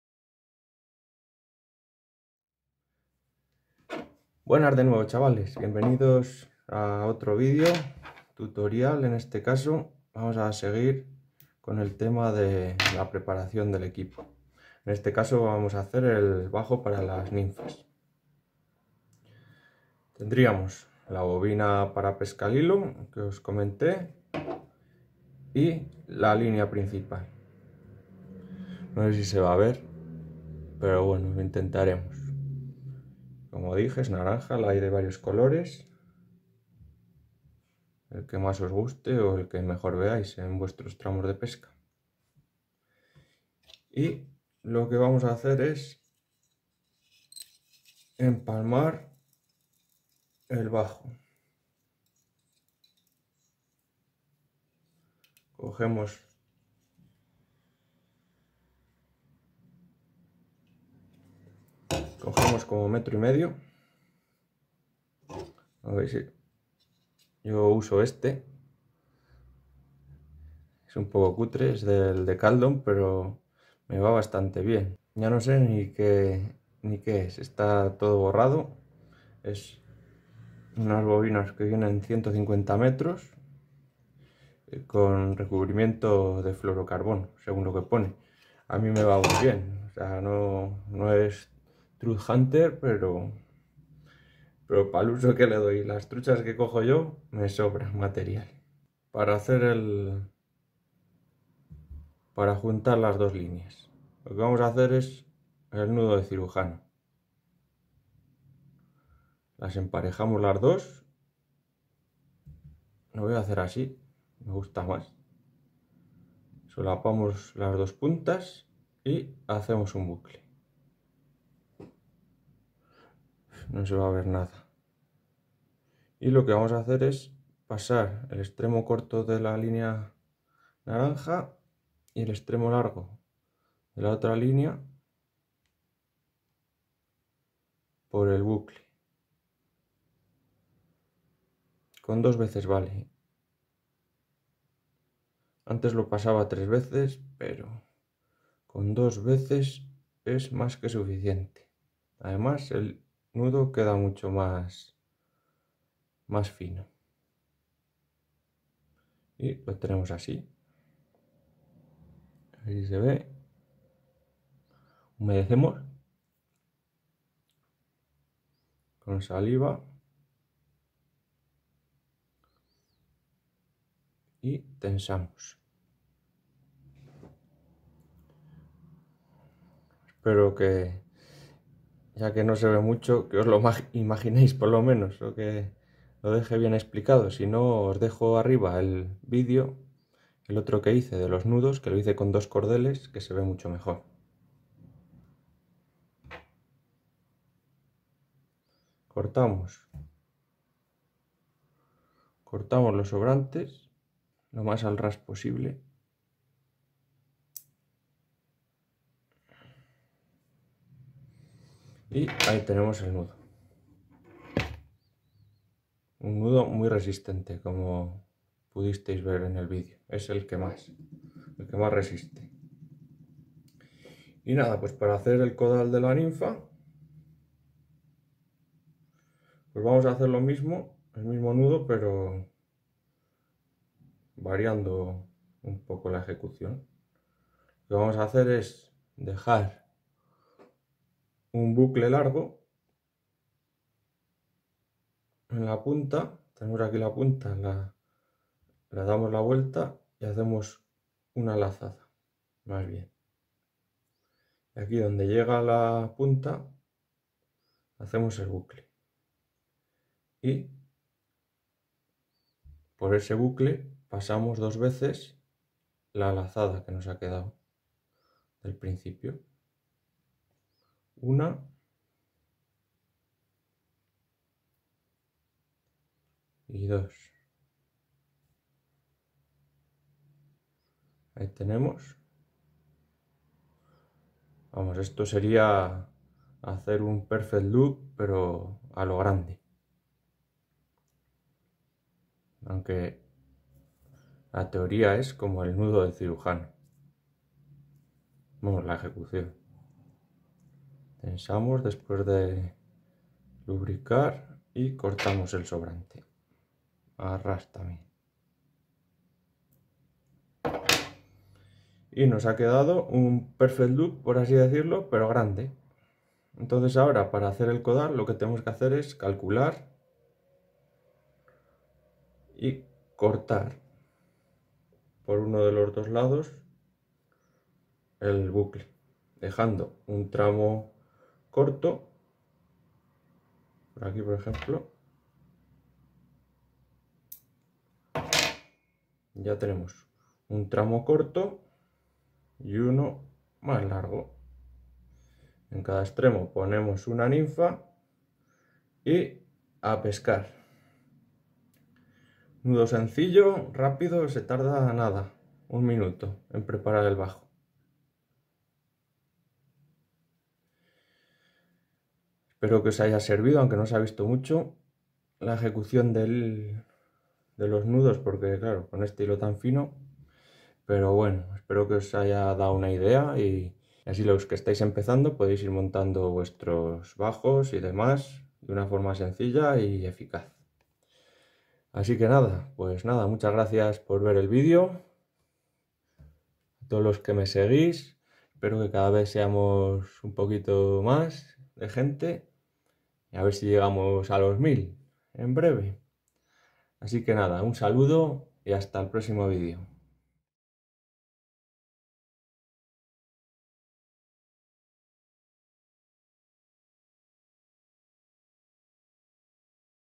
Tutorial 3 de 3: Como hacer un bajo para ninfas (pesca al hilo)
Esta es la segunda parte del tutorial, en esta ocasión os explico cómo hacer un bajo sencillo para ninfas o para pescar al hilo. Quedaros con los truquillos, el nudo en sí no se ve muy bien porque queda desenfocado, pero podeis ver como se hace en el tutorial que hice de nudos anteriormente